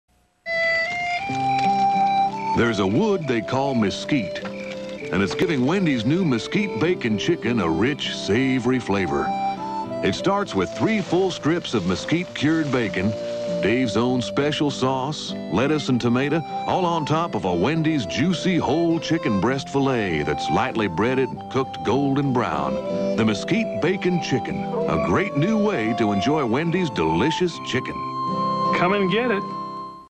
1995 Wendy's Mesquite Bacon Chicken Sandwich TV Commercial
The commercial was originally aired on June 18th,1995. It was run during the "60 Minutes" on KPHO Channel 5 in Phoenix AZ. KPHO Channel 5 was the CBS in Phoenix in 1995.